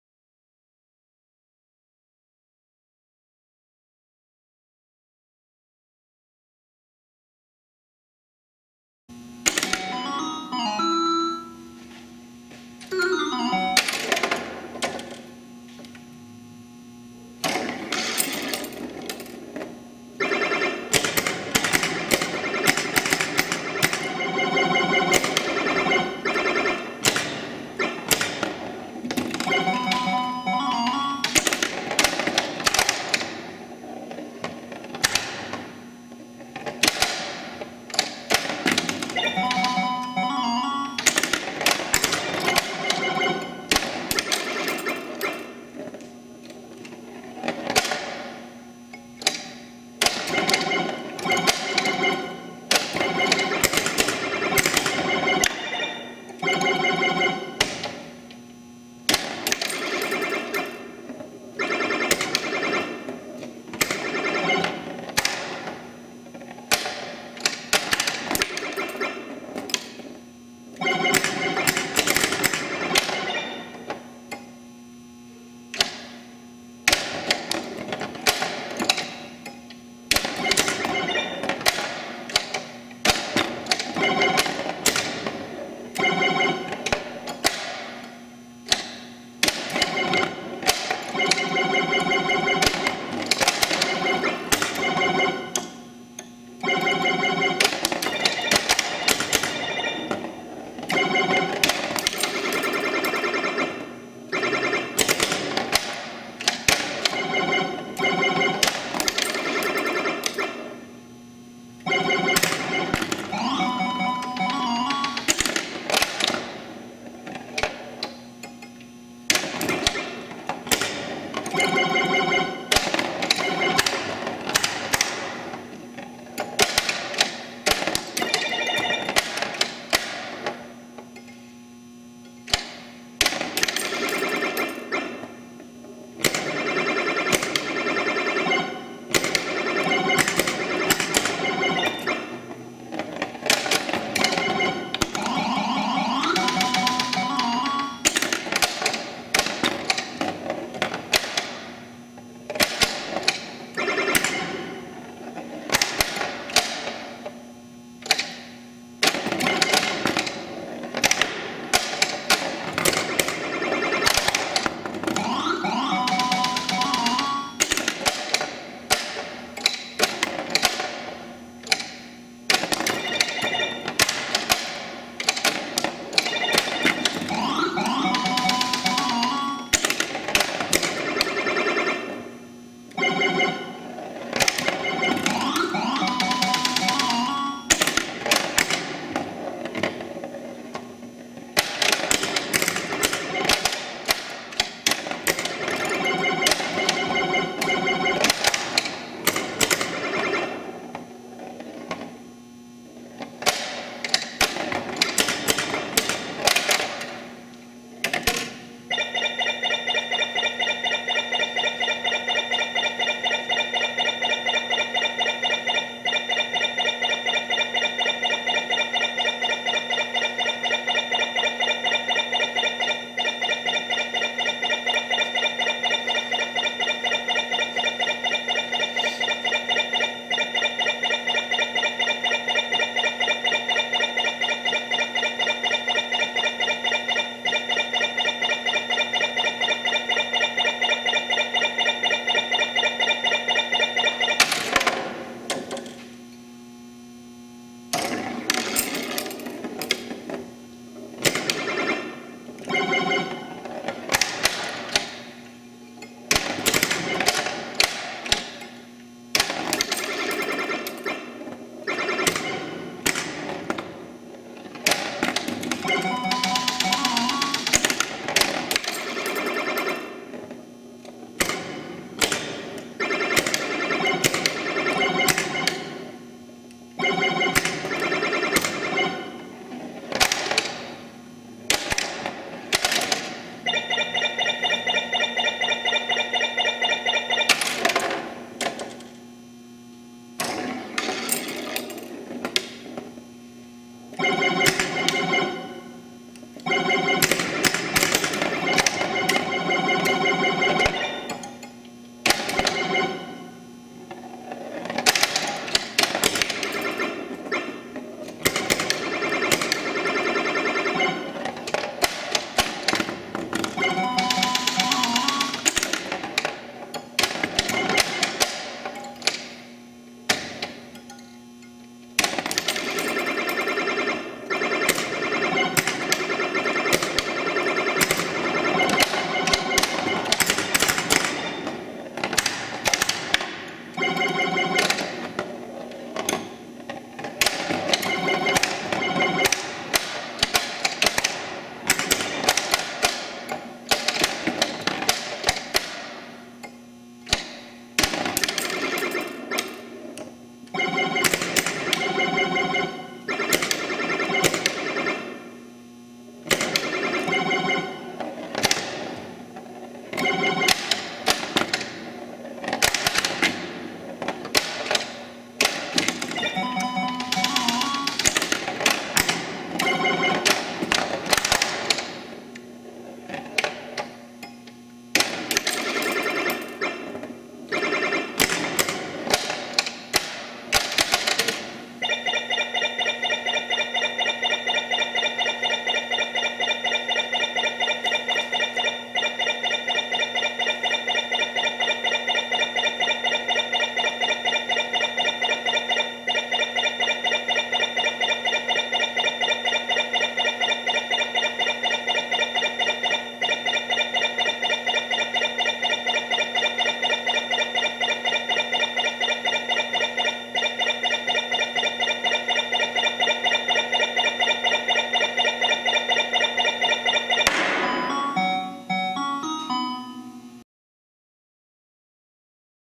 Lost World Pinball Gameplay
Pinball gameplay filmed at PAPA Headquarters.